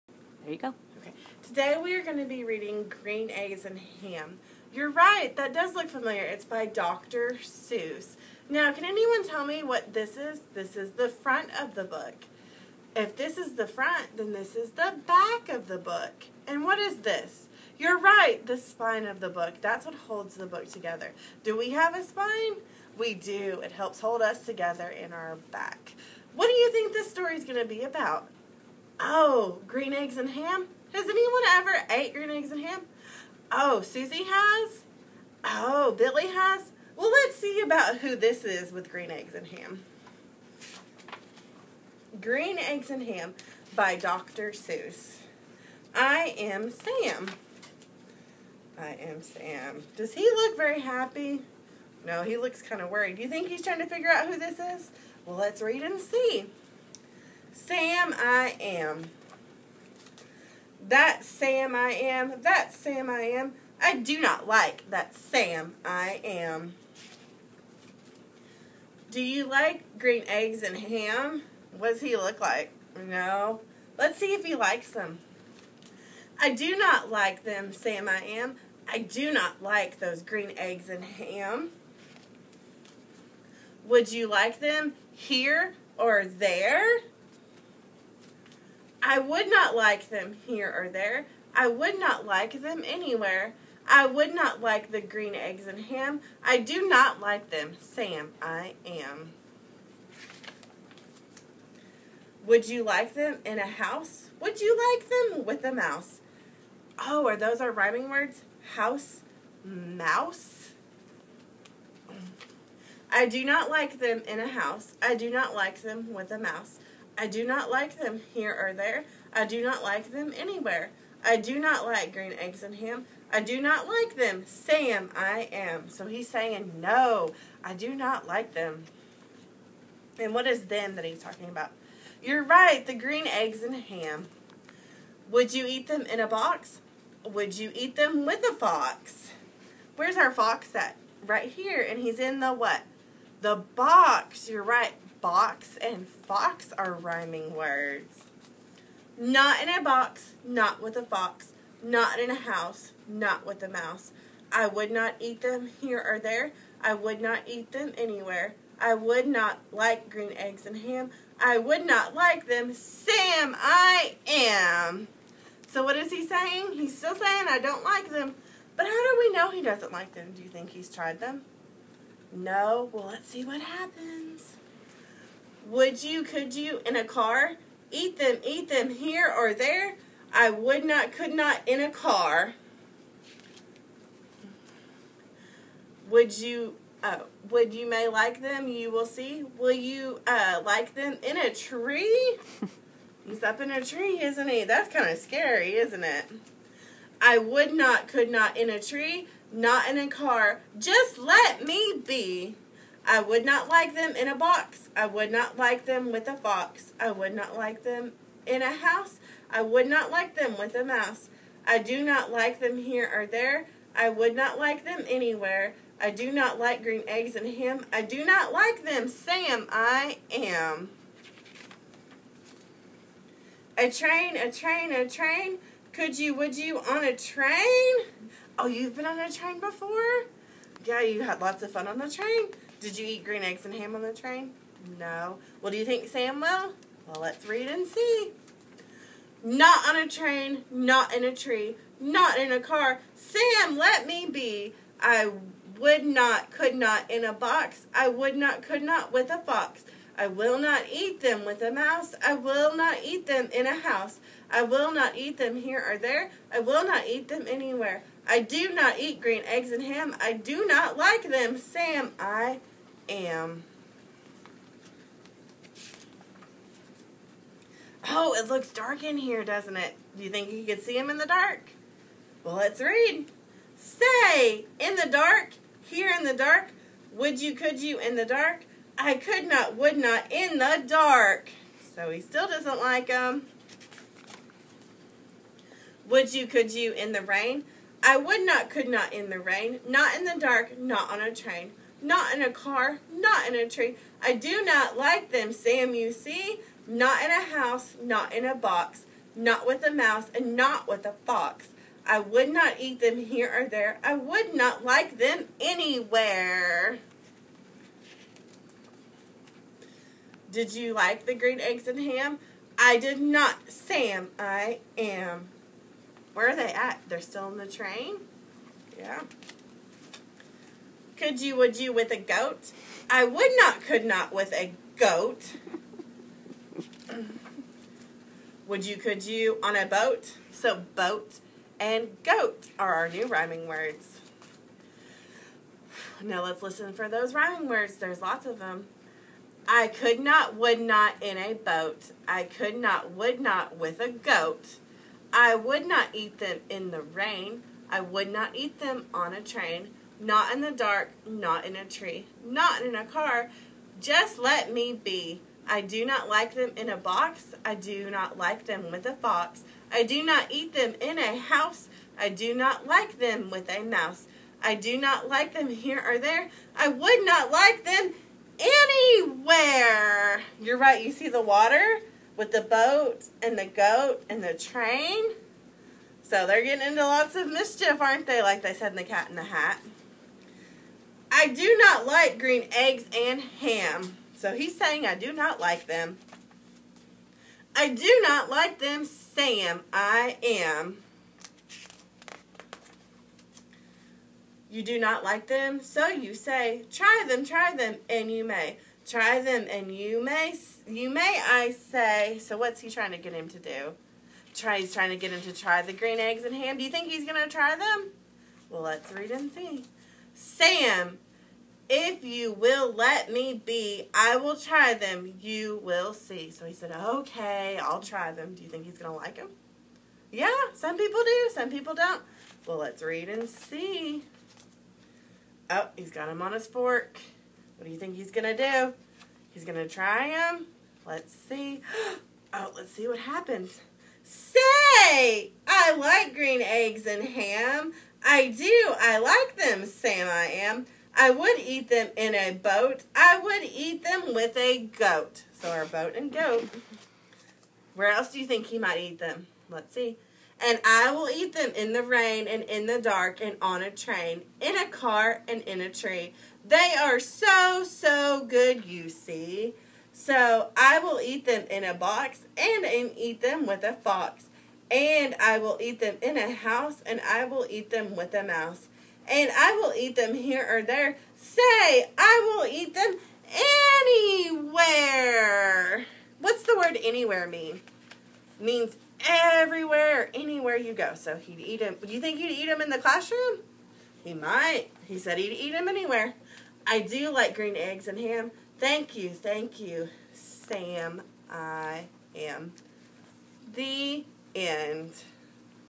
CADC Shannon HIlls Head Start/ABC - Ms. Amanda - "Green Eggs and Ham"
Ms. Amanda reading "Green Eggs and Ham" by Dr. Seuss